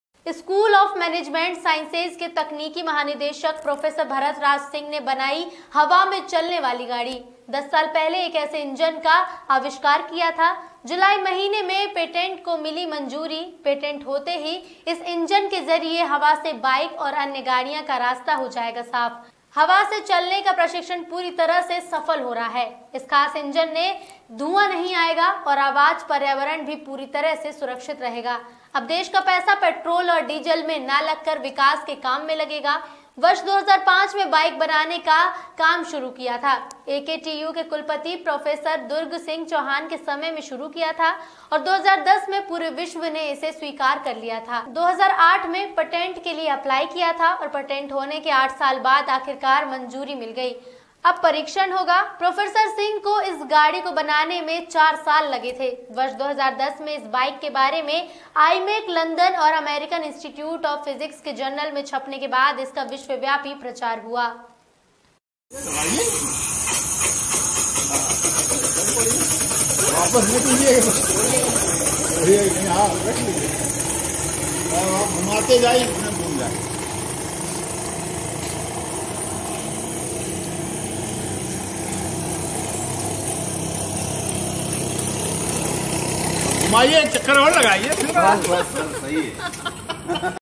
News Network 24-अब हवा से चलेगी आपकी गाड़ी-(23-09-2020) at 12:30 AM
स्कूल ऑफ मैंनेजमेंट साइंसेज के महानिदेशक प्रोफ. भरत राज  सिंह ने बनाई  हवा से चलने वाली मोटर-बाइक।
उक्त ऐअर-ओ-बाइक का प्रदर्शन टाटा मोटर्स, लखनऊ में 15 सितम्बर 2020 को किया गया जिसके इंजीनियर्स ने इसक़े  तकनीक की सराहना की ।